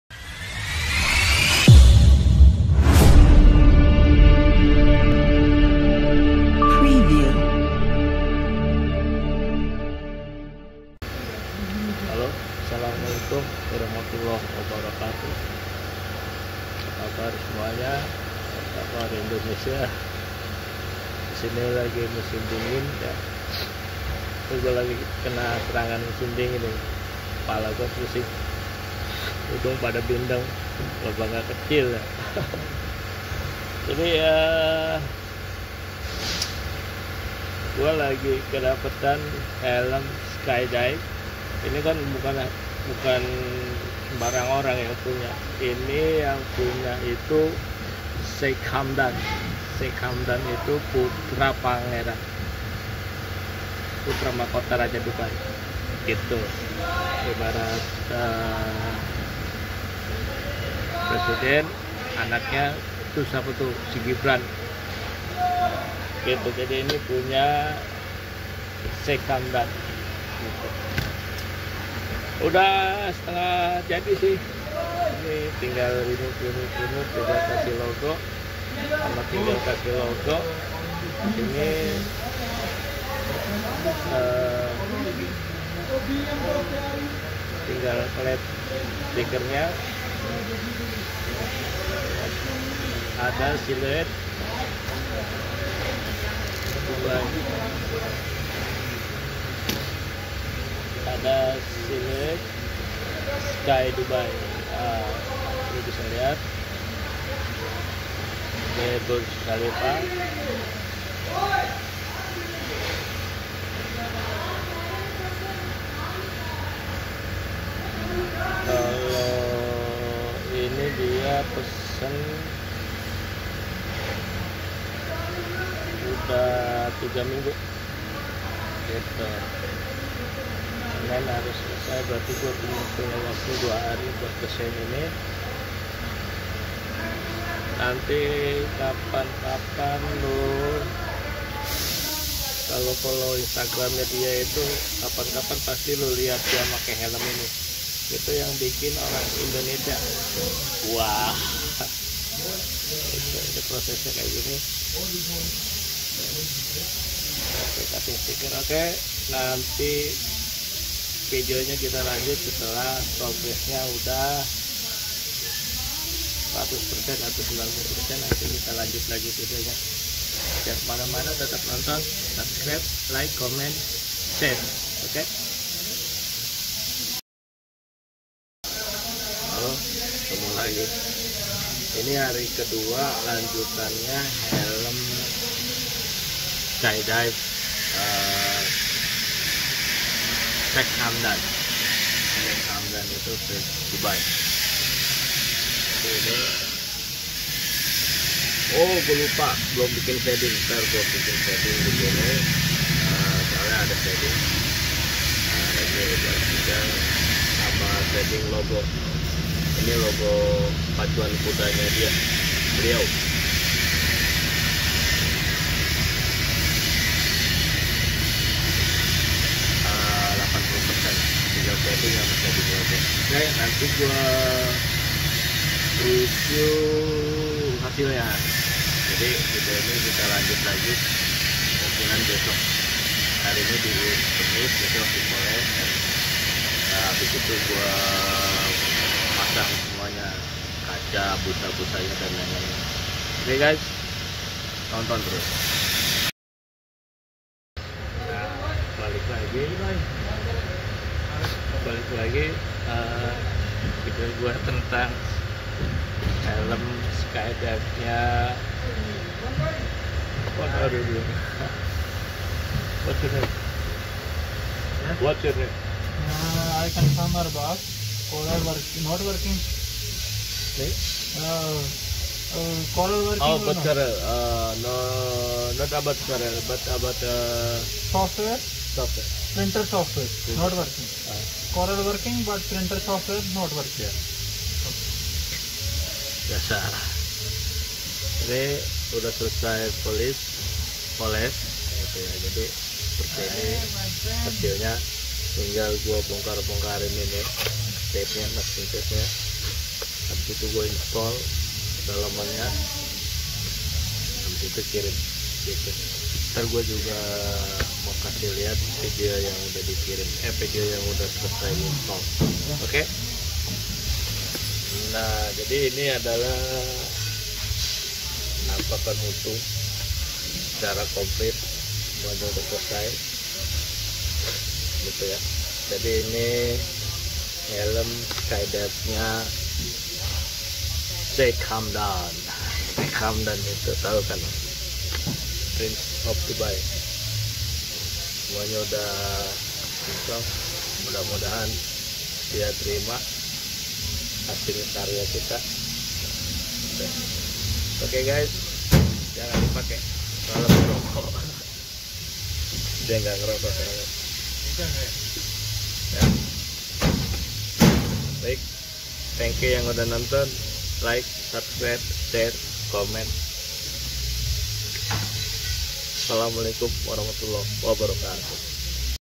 airbrush helm skydive punya prince of DUBAI
Sheikh Hamdan bin Mohammed bin Rashid Al Maktoum merupakan putra mahkota Dubai. Pria 34 tahun ini adalah anak kedua dari Sheikh Mohammed bin Rashid Al Maktoum yang merupakan wakil presiden Uni Emirat Arab dan juga pemimpin Dubai.

Bagaimana tidak, kepada 5,1 juta pengikutnya di Instagram, pria yang kerap di sapa Fazza ini juga suka menunjukkan kemewahan hidupnya sebagai seorang putera raja.
weellllll... bangga beud rasanya 🤭🥰🥰